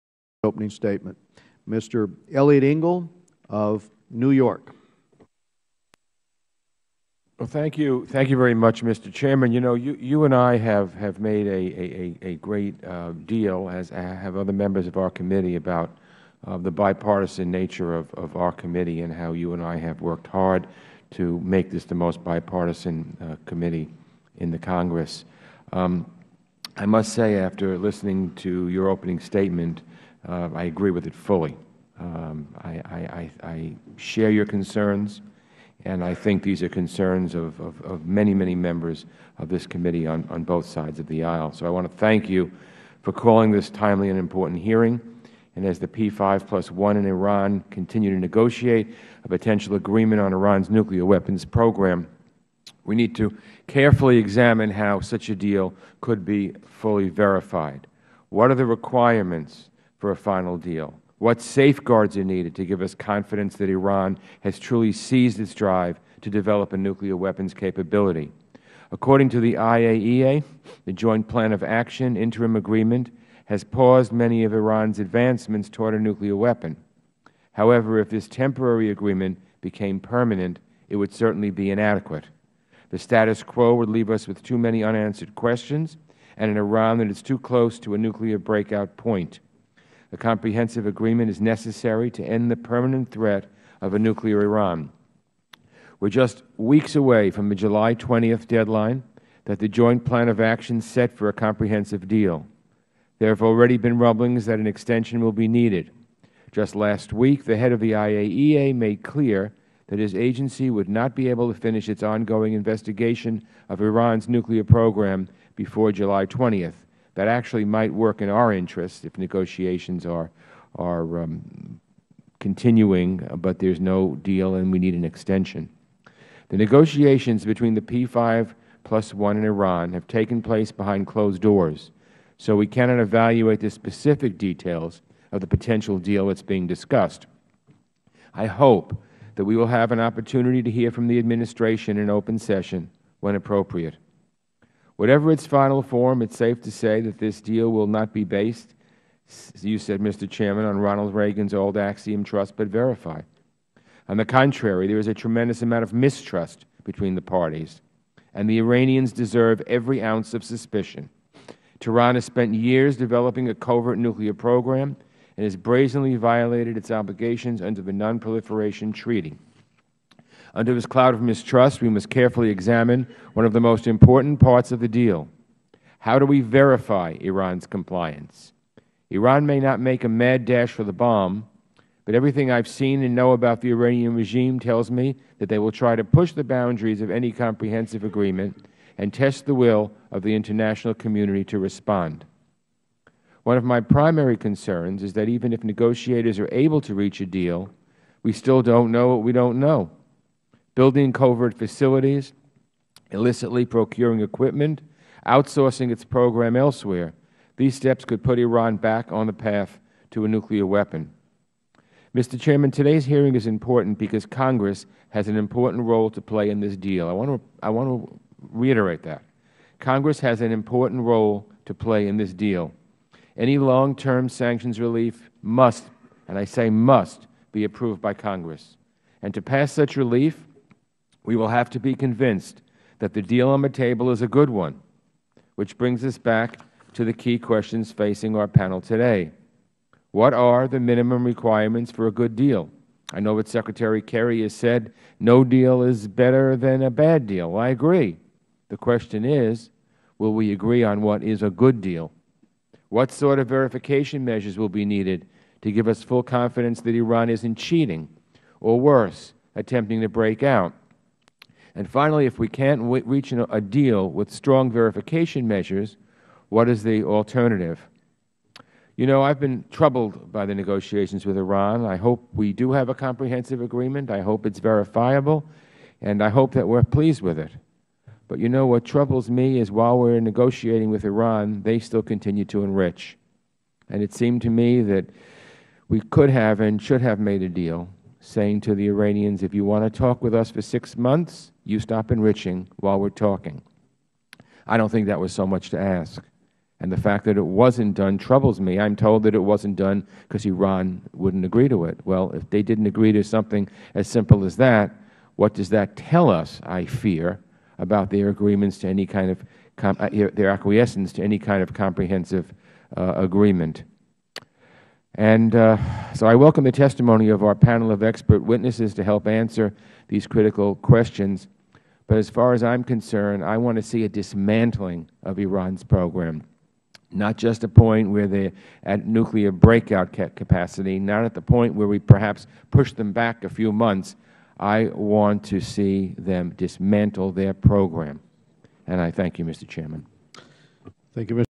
6 10 14 Mr Engel Opening Statement Full Committee Hearing " "Verifying Iran's Nuclear Compliance"
Ranking Member Eliot L. Engel's opening statement at full committee hearing "Verifying Iran's Nuclear Compliance."